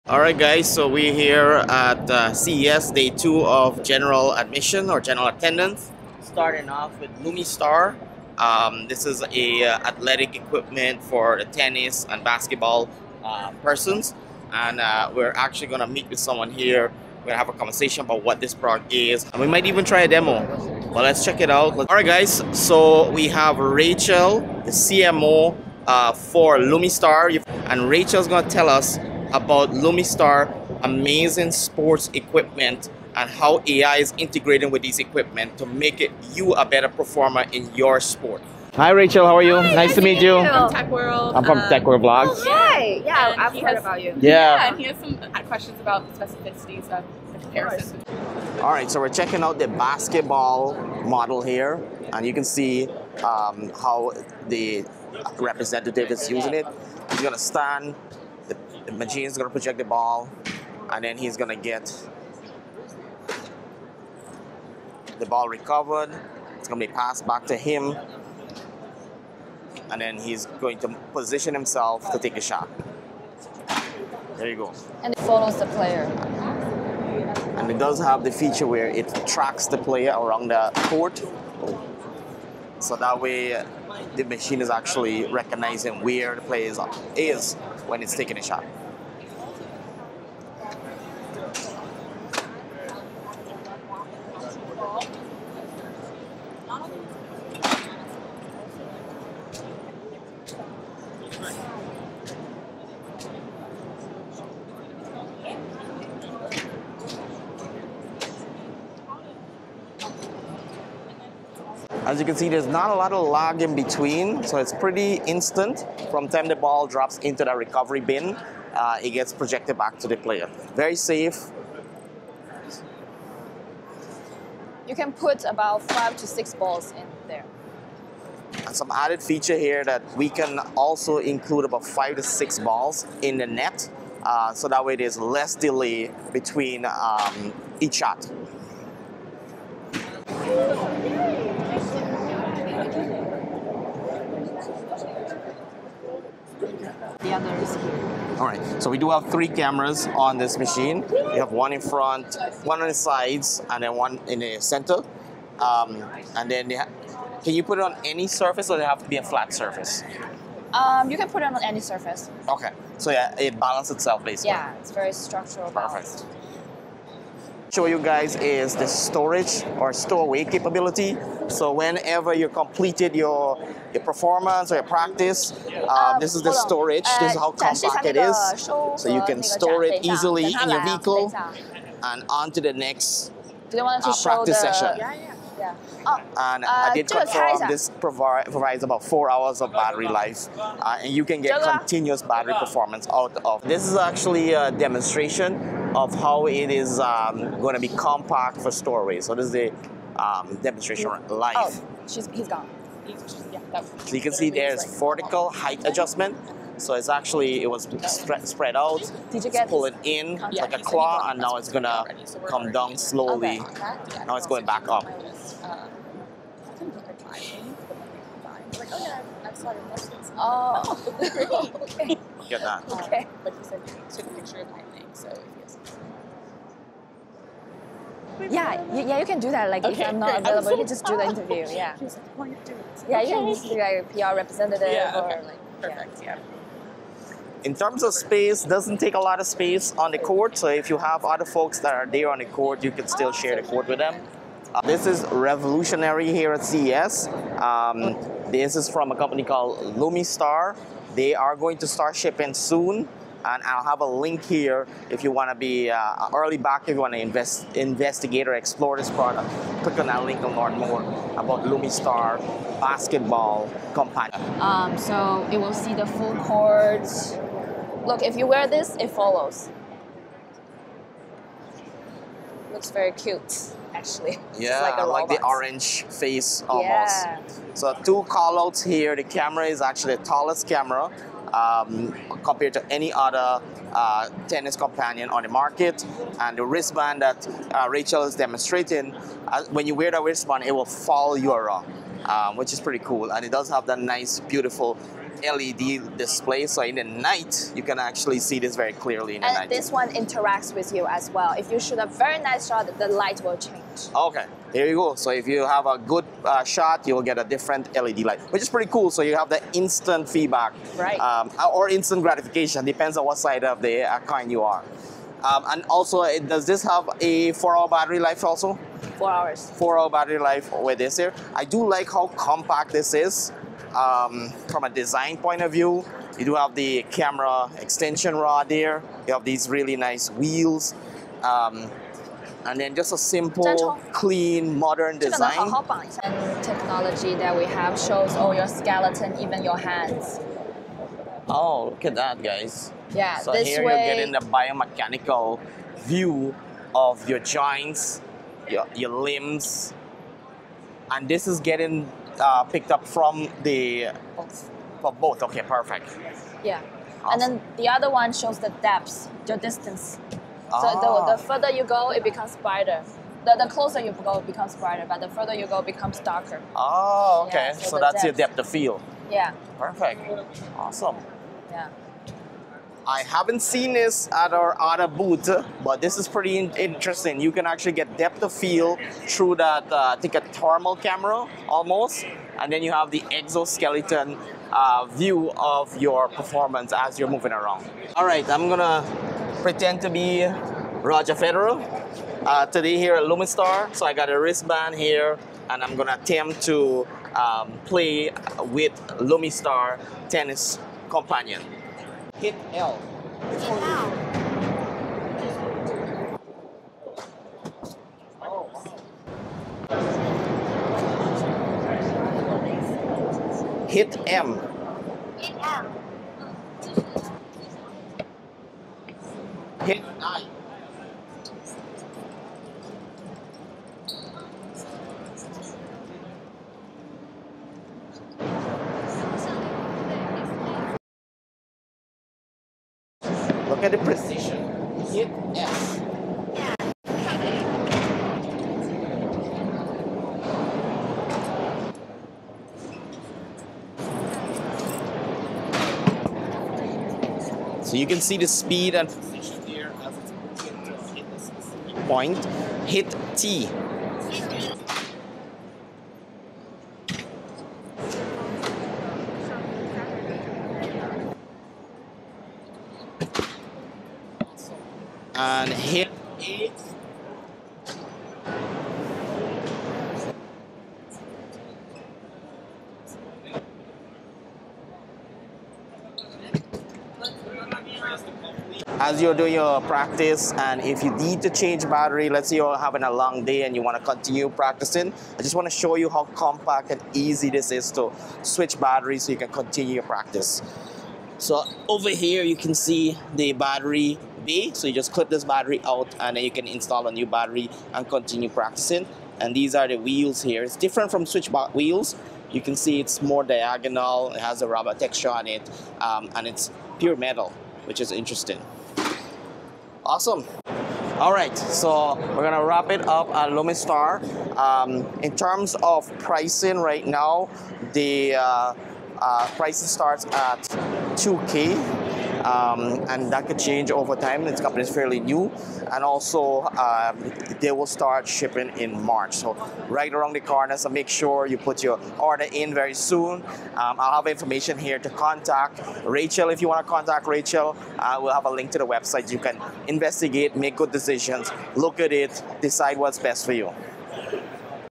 CES 2026: Lumistar’s AI Sports Tech Is INSANE (Tero, Tero Pro & Carry)
At CES 2026, Lumistar unveiled AI-powered sports robots that could change how athletes train forever…
In this video, I go hands-on with Lumistar’s latest AI sports training technology, including the Tero, Tero Pro, and Carry systems, showcased live at CES 2026. These intelligent robots combine computer vision, real-time analytics, ball tracking, and automated drills to help athletes train smarter, faster, and more efficiently than ever before.
From basketball shooting automation to tennis stroke analysis and ball delivery, Lumistar’s AI systems are designed for players, coaches, academies, and elite training facilities looking to gain a competitive edge using data-driven performance insights.
🎯 What you’ll see in this video:
Live demo of Lumistar Tero & Tero Pro in action
How Lumistar’s AI vision and tracking works in real time
Why this tech stood out at CES 2026
Use cases for athletes, trainers, and sports programs
My first impressions on whether this is the future of sports training
If you’re into AI, robotics, sports tech, CES coverage, or the future of training, this is a must-watch.